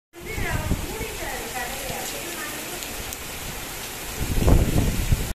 rain in chennai
Finally rain in Chennai due to asanicyclone.